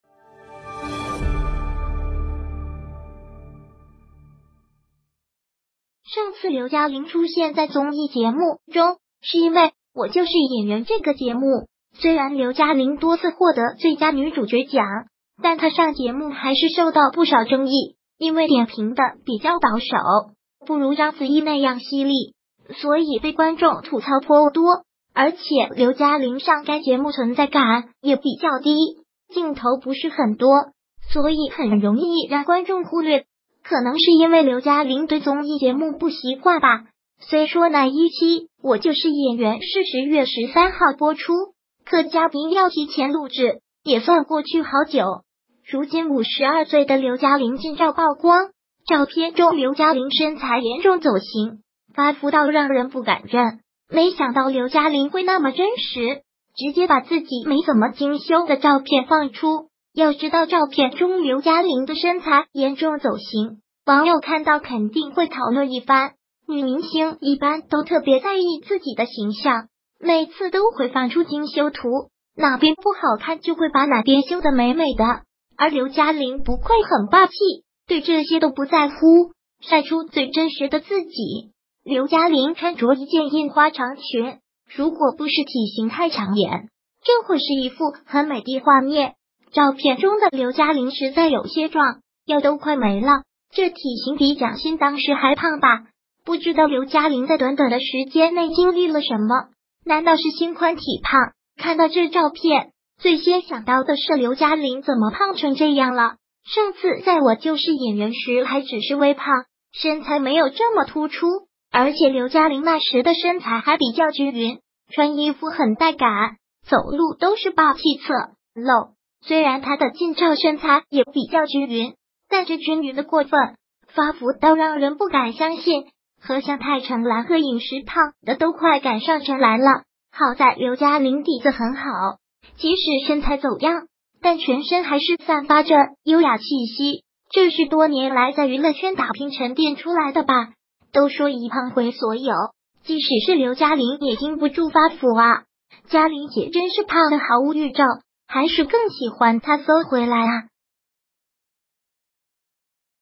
52岁的刘嘉玲近照曝光，身材严重走形，发福到不敢认
上次刘嘉玲出现在综艺节目中是因为《我就是演员》这个节目，虽然刘嘉玲多次获得最佳女主角奖，但她上节目还是受到不少争议。因为点评的比较保守，不如章子怡那样犀利，所以被观众吐槽颇多。而且刘嘉玲上该节目存在感也比较低，镜头不是很多，所以很容易让观众忽略，可能是因为刘嘉玲对综艺节目不习惯吧。虽说那一期《我就是演员》是十月13号播出，可嘉宾要提前录制，也算过去好久。如今52岁的刘嘉玲近照曝光，照片中刘嘉玲身材严重走形，发福到让人不敢认。没想到刘嘉玲会那么真实，直接把自己没怎么精修的照片放出，要知道照片中刘嘉玲的身材严重走形，网友看到肯定会讨论一番。女明星一般都特别在意自己的形象，每次都会放出精修图，哪边不好看就会把哪边修的美美的。而刘嘉玲不愧很霸气，对这些都不在乎，晒出最真实的自己。刘嘉玲穿着一件印花长裙，如果不是体形太抢眼，这会是一副很美的画面。照片中的刘嘉玲实在有些“壮”，腰都快没了，这体形比蒋欣当时还胖吧。不知道刘嘉玲在短短的时间内经历了什么，难道是心宽体胖？看到这照片，最先想到的是刘嘉玲怎么胖成这样了？上次在《我就是演员》时还只是微胖，身材没有这么突出。而且刘嘉玲那时的身材还比较均匀，穿衣